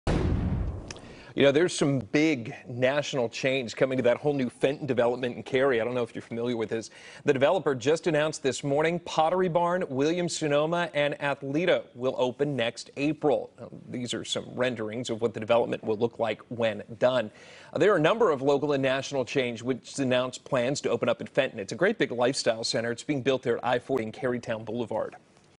National chains coming to Fenton development in Cary
Four new businesses were added to the roster at Cary's under-construction Fenton development.